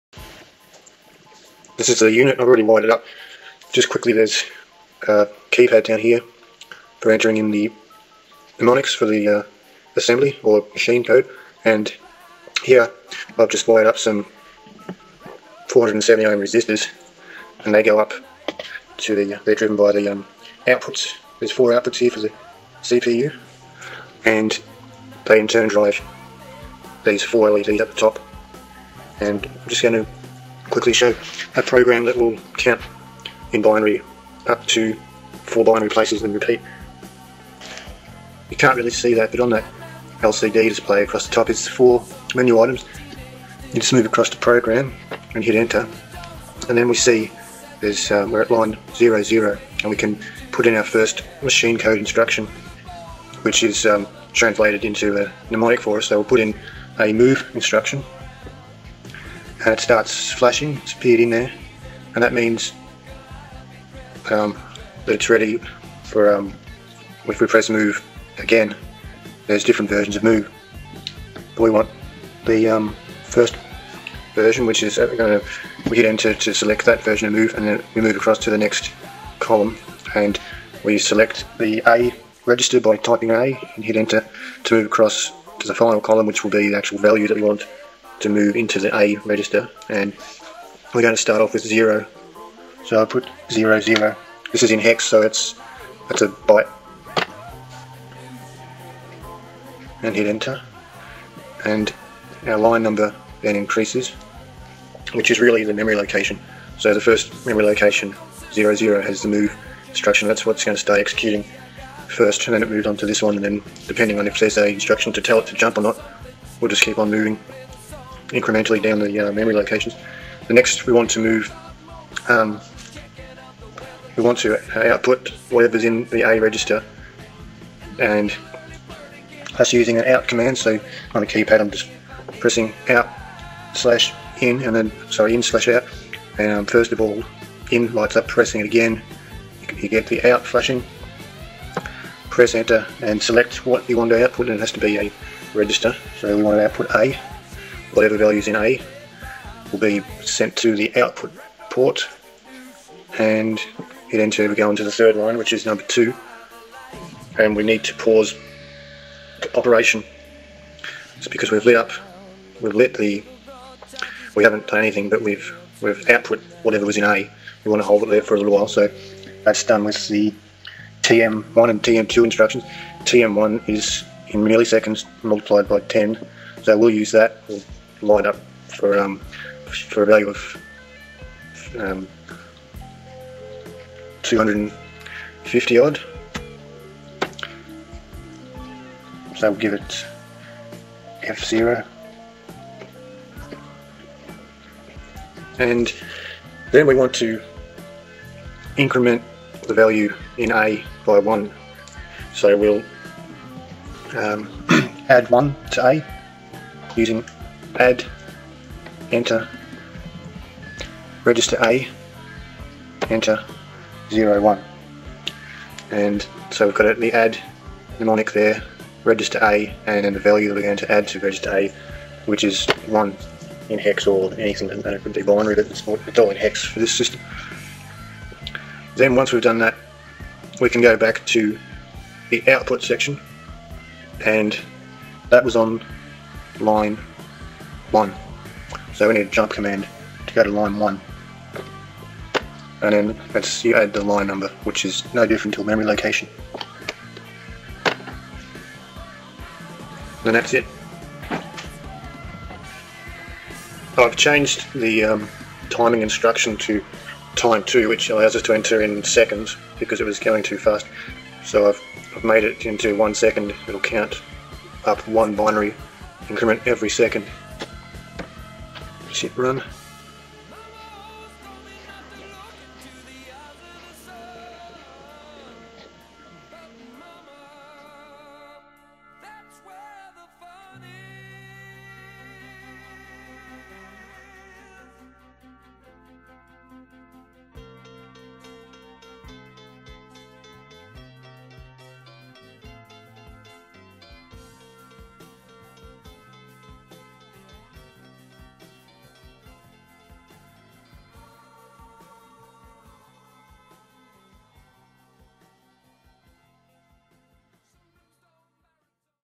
Programming the 500 in 1 lab
A quick example, showing binary addition. This program is only 5 lines.

00 MOV A,00h
01 OUT A
02 TM2 01h
03 ADD A,01h
04 JMP 01h